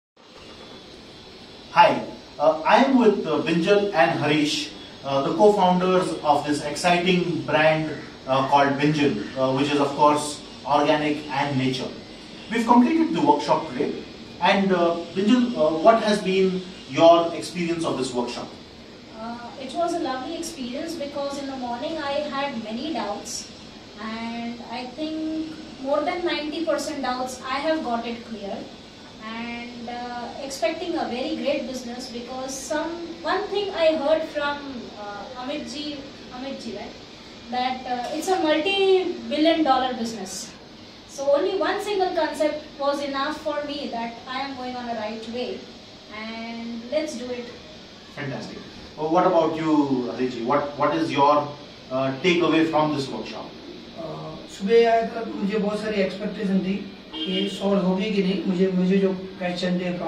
Franchise your Organic and Natural Clothing Brand
An organic and natural clothing brand based in Mumbai franchising their business in India. Grab the opportunity to be part of this brand by taking up the franchise.
Connect to sparkle★minds today.